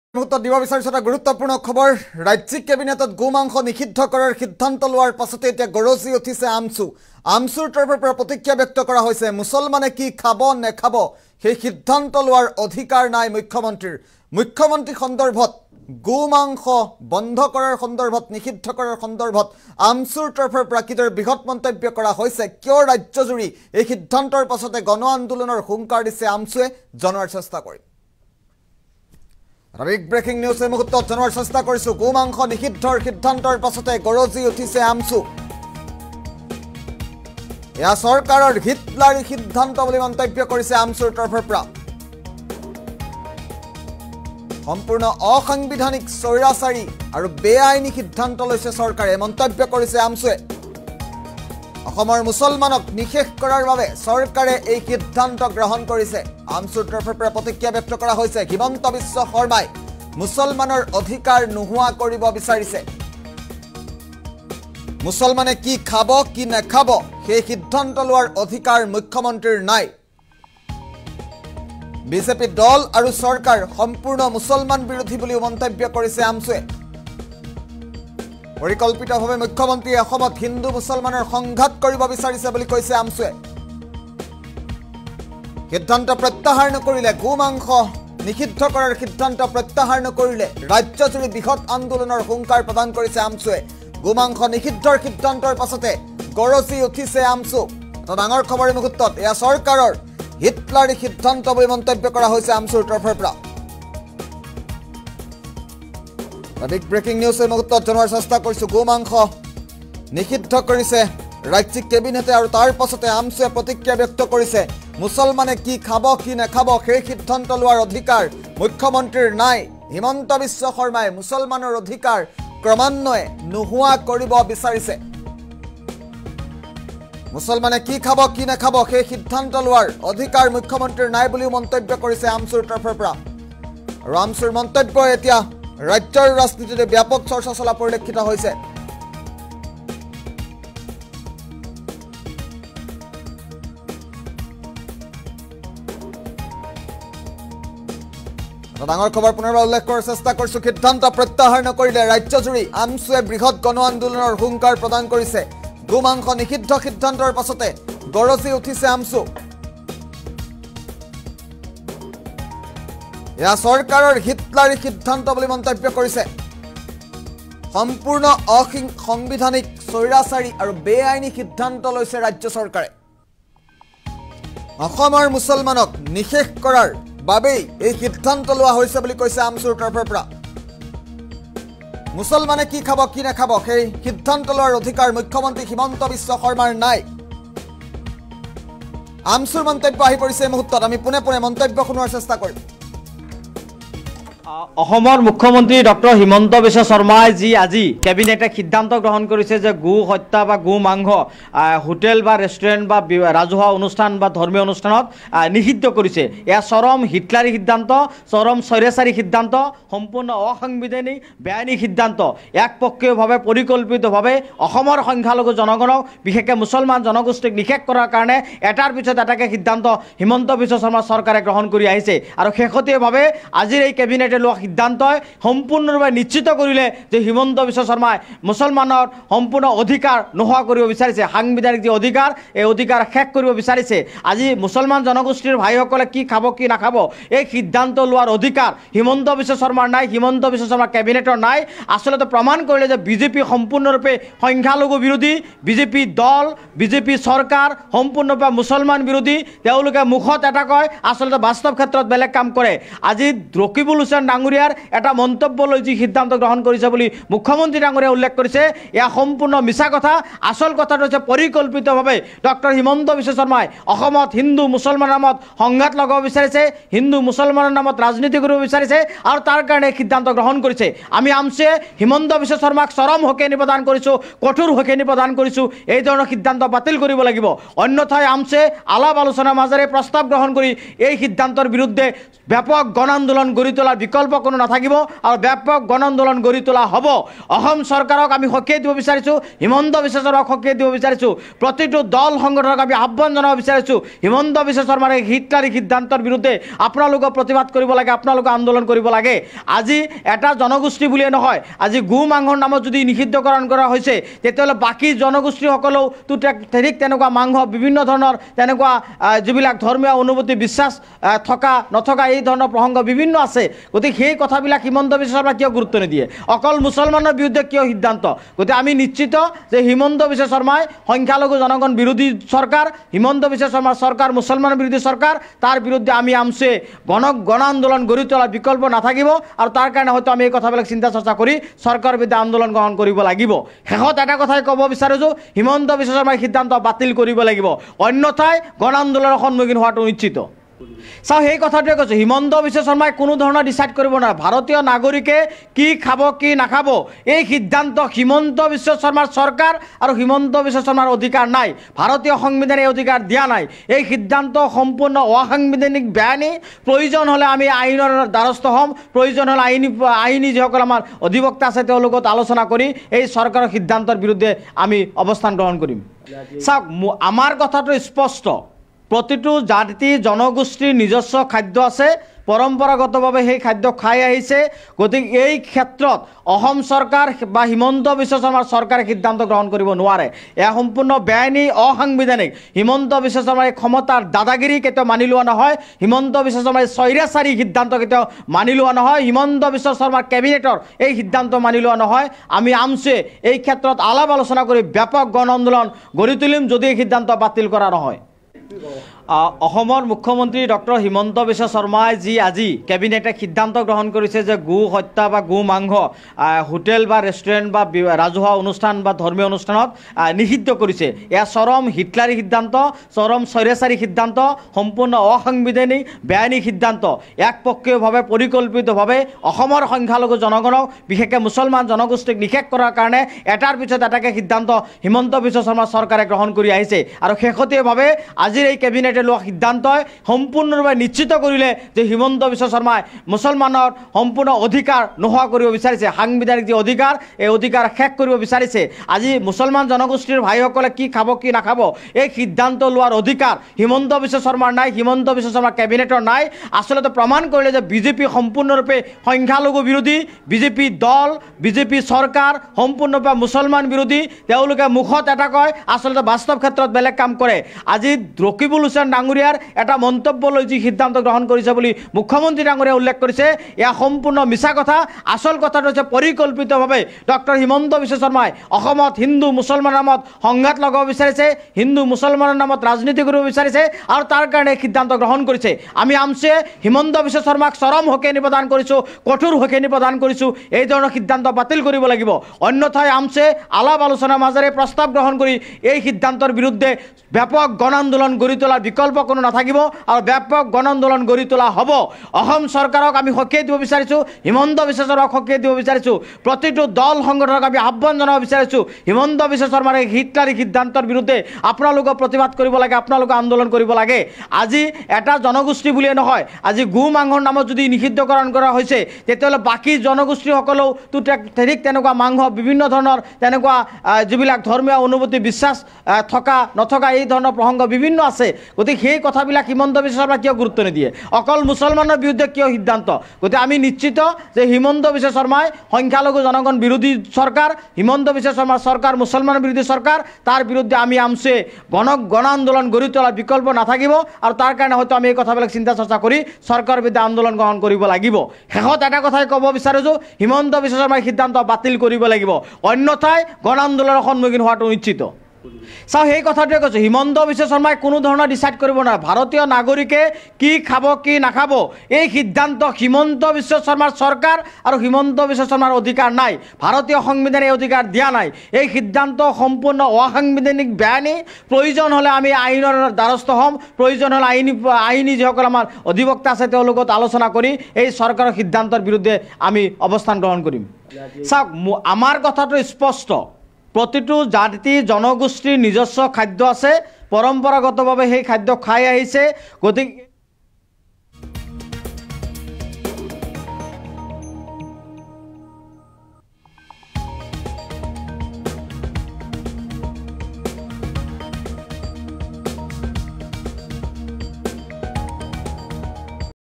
মুছলমানে কি খাব, নাখাব সেই সিদ্ধান্ত লোৱাৰ অধিকাৰ নাই মুখ্যমন্ত্ৰীৰ।
মুছলমানে কি খাব, নাখাব সেই সিদ্ধান্ত লোৱাৰ অধিকাৰ নাই মুখ্যমন্ত্ৰীৰ। বিশেষ মাংস নিষিদ্ধৰ সিদ্ধান্ত প্ৰত্যাহাৰ নকৰিলে ৰাজ্যজুৰি বৃহৎ আন্দোলনৰ হুংকাৰ আমছুৰ।